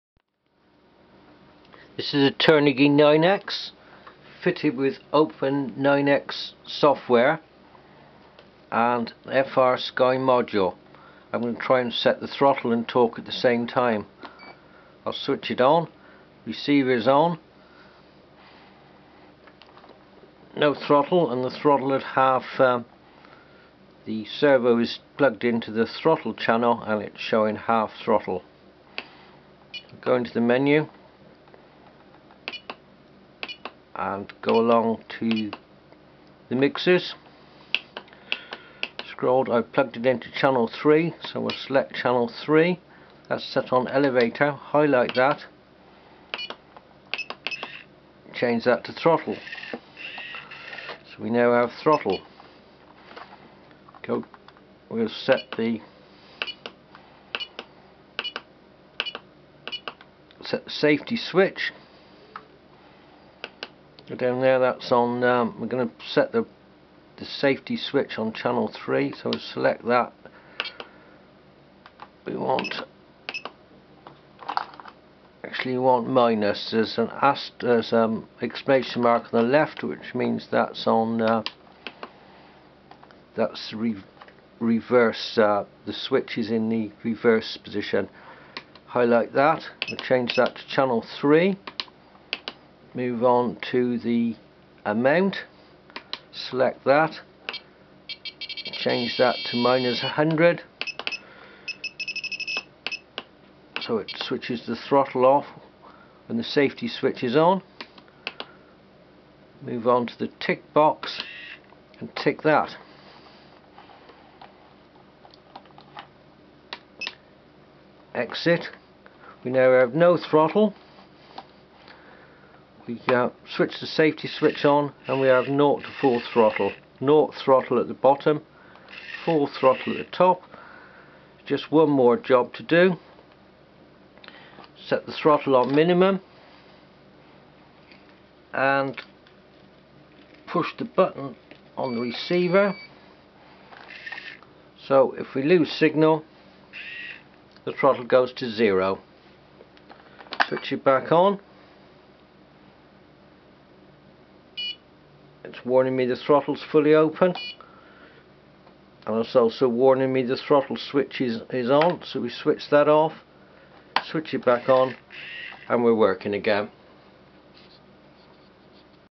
Turnigy 9x Open9x throttle setting
Setting the throttle with Open9X software with safety switch and failsafe on a Turnigy 9X